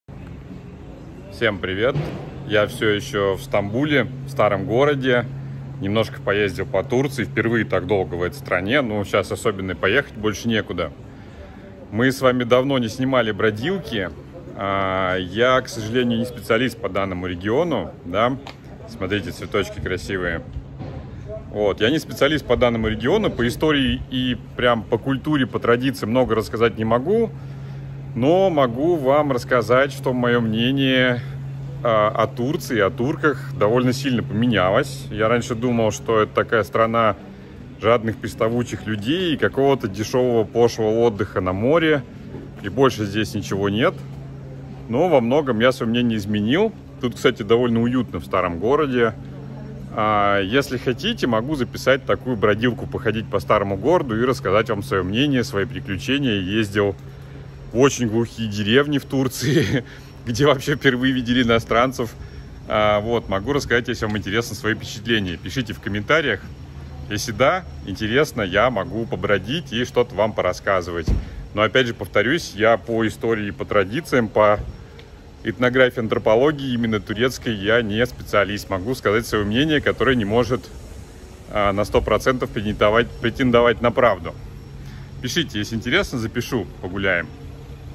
Пойдём гулять? • Стамбул 2020
Я раньше считал Турцию страной навязчивого сервиса и отдыха низкого качества. Но погулял по Стамбулу за последнее время и мое мнение о стране и о турках сильно поменялось.

Хотите виртуальную прогулку? Покажу и расскажу, на что обратил внимание. И не только.